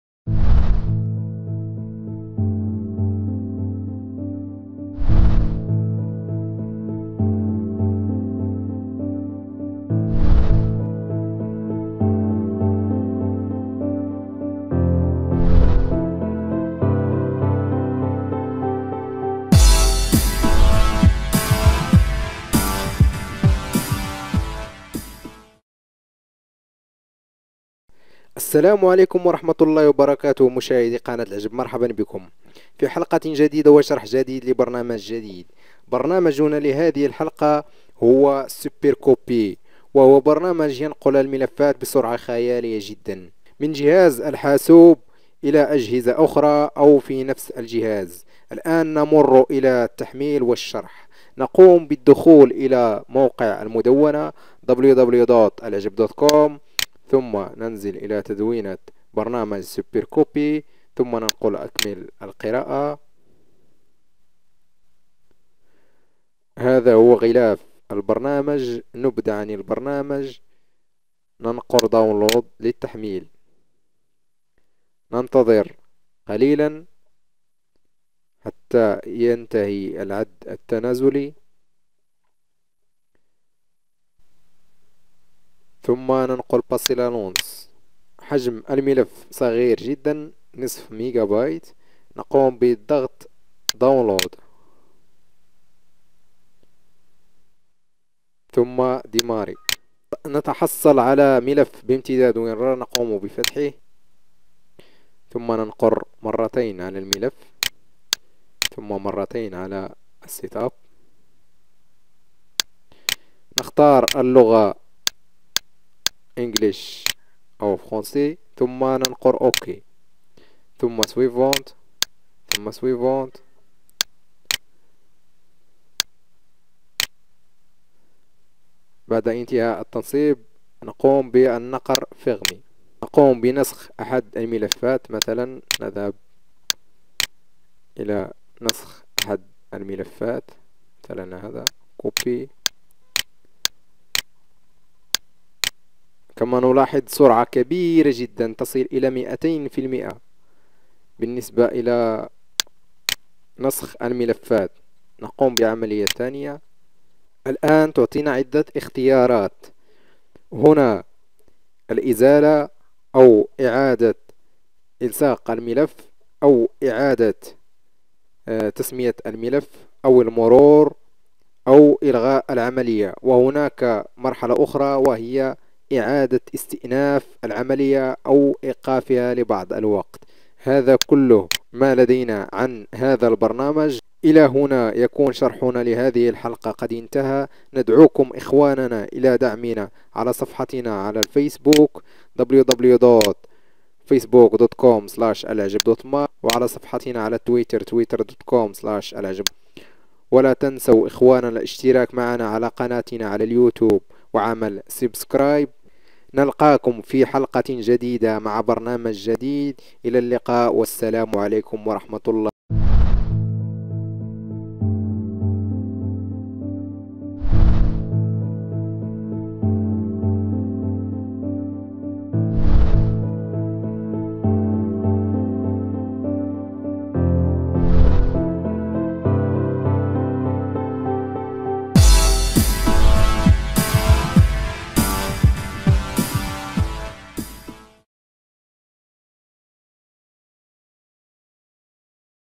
الحلقة 16 : شرح برنامج SuperCopier2 لنسخ ملفاتكم بسرعة خيالية
رنامج SuperCopier2 هو عبارة عن برنامج نقل سريع من جهاز الكمبيوتر الى اجهزة اخرى مثل الفلاشة وكارت الميمورى وهارد دسك وغيره كما انة ينسخ ملفات الـ CD على الجهاز بسرعة فائقة وايضا يستطيع البرنامج نقل ونسخ من جهاز كمبيوتر الى جهاز كمبيوتر اخر.